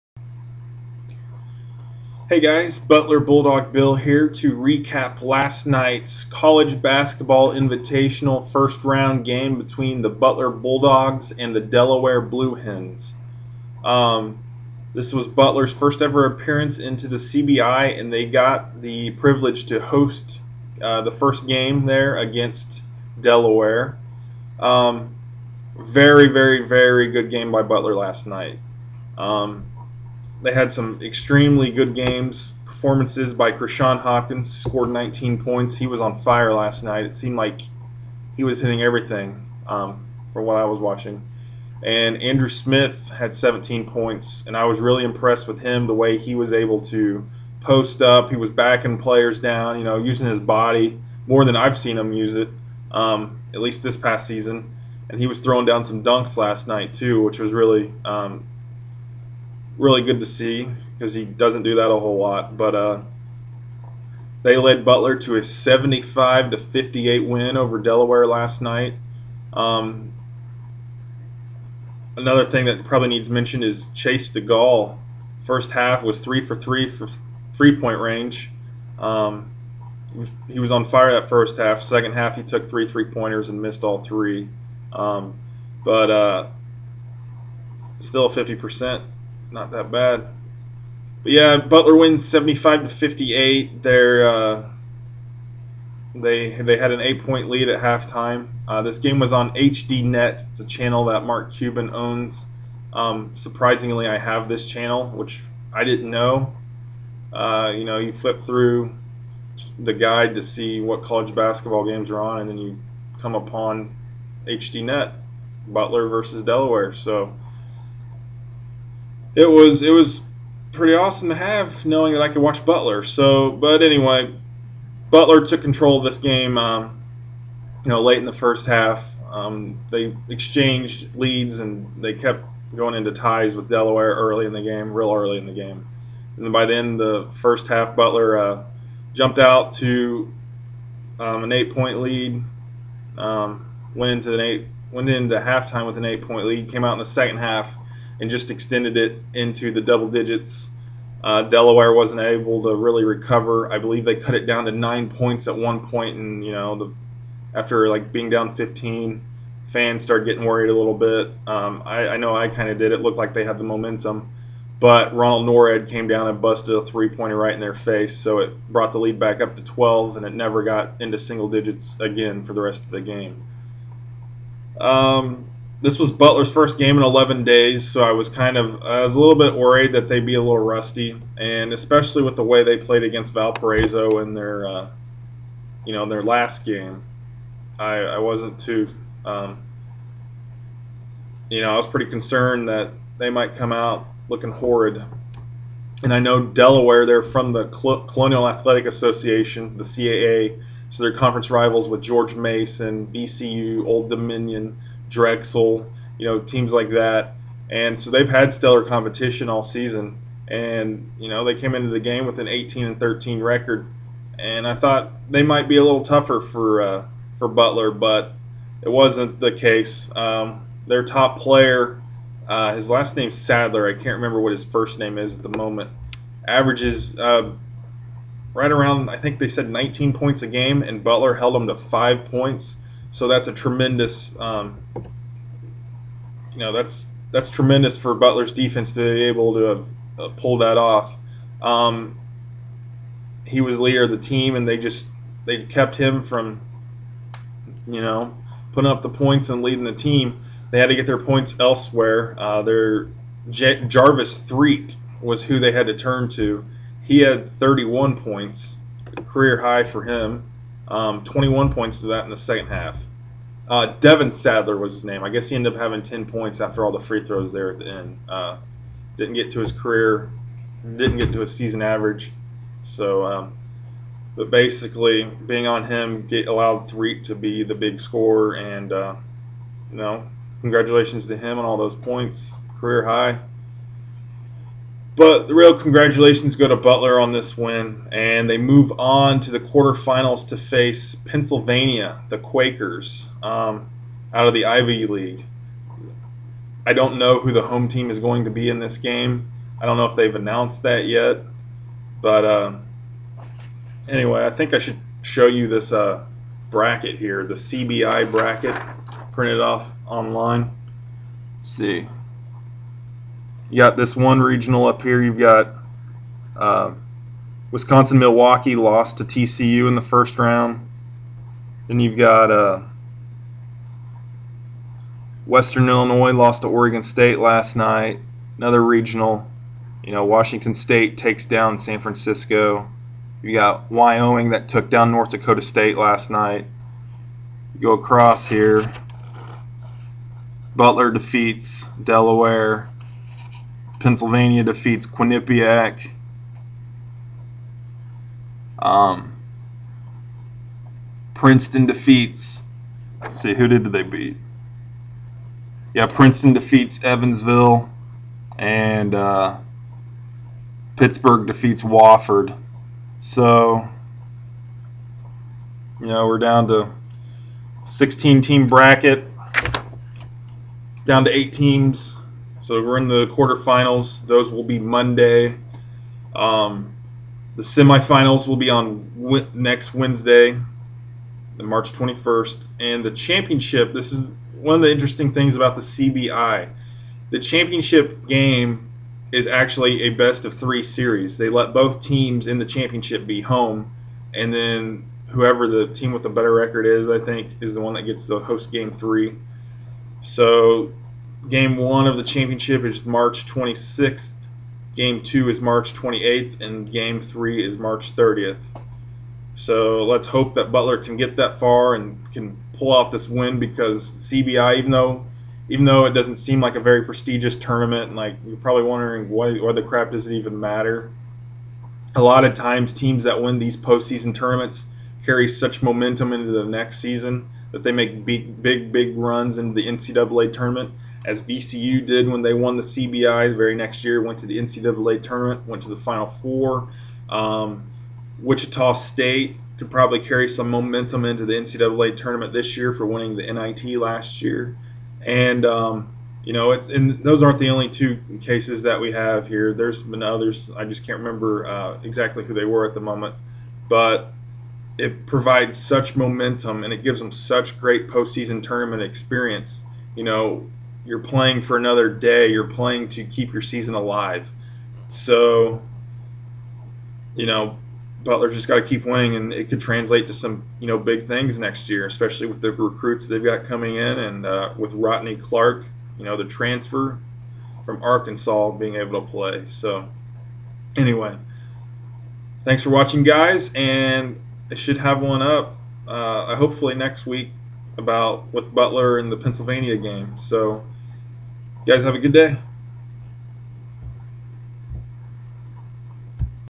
Delaware at Butler Recap (College Basketball Invitational: First Round)
Recap and analysis of the Butler Bulldogs' 75-58 win against the Delaware Blue Hens in the first round of the College Basketball Invitational.